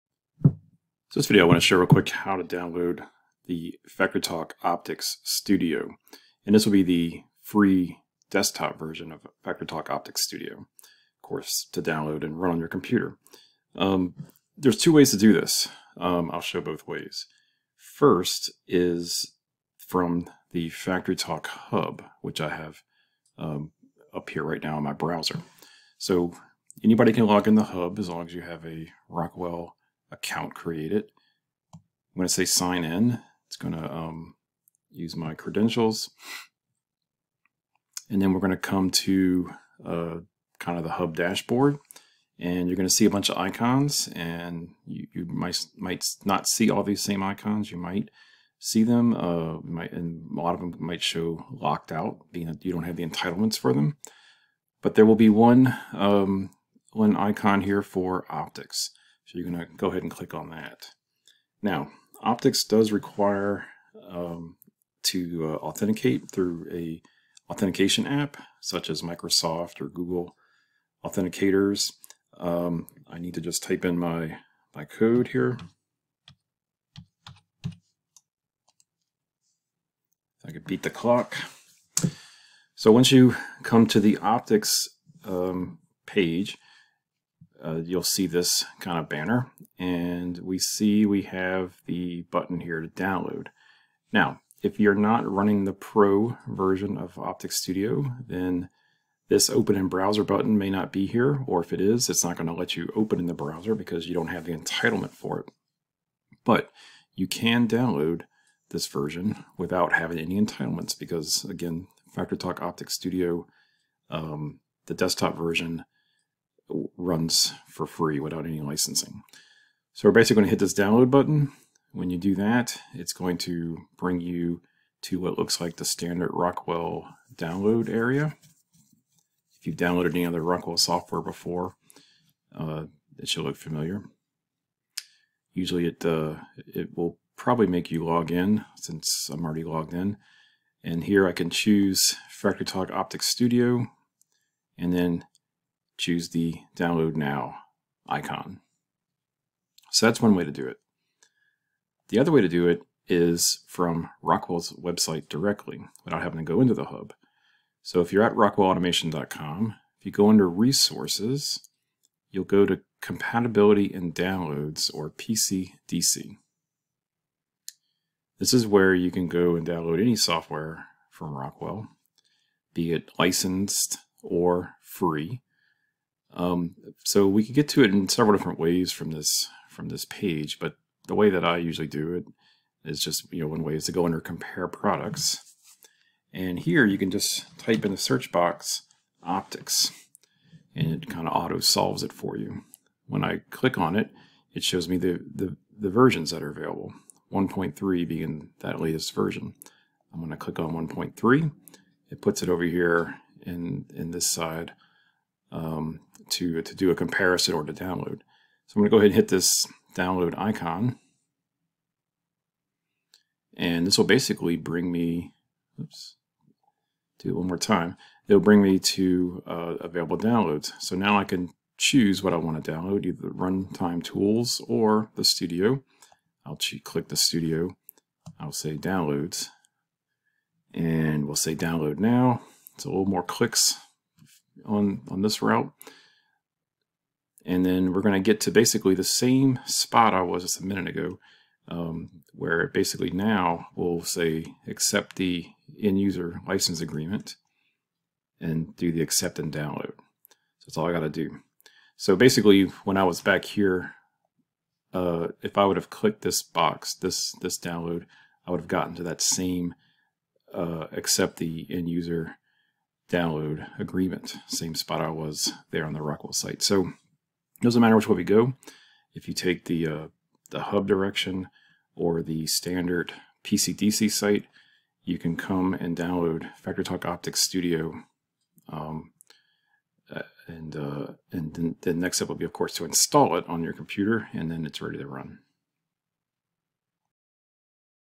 How to Download FactoryTalk Optix Studio
This video shows how to download FactoryTalk Optix Studio from the FactoryTalk Hub as well as Rockwell Automation's Product Compatibility and Download Center (PCDC).

0:18 Download from FactoryTalk Hub
2:32 Download from Rockwell's Product Compatibility and Download Center (PCDC)